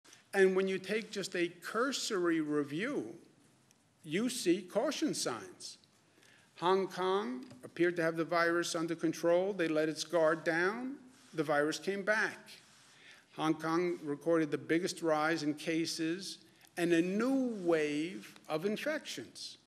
Cuomo: Look At Other Countries' Second Wave
ALBANY – New York Governor Andrew Cuomo warns reopening the U.S. Economy too soon could lead to a second wave of Coronavirus infections.

He said the nation should look at other nations and regions that have gone through the "reopening process."

"When you take just a cursory review, you see caution signs,” said Gov. Andrew Cuomo. “Hong Kong appeared to have the virus under control. They let its guard down, the virus came back. Hong Kong reported the biggest rises in cases in a new wave of infections.”

The Governor also pointed to China, Singapore and Italy.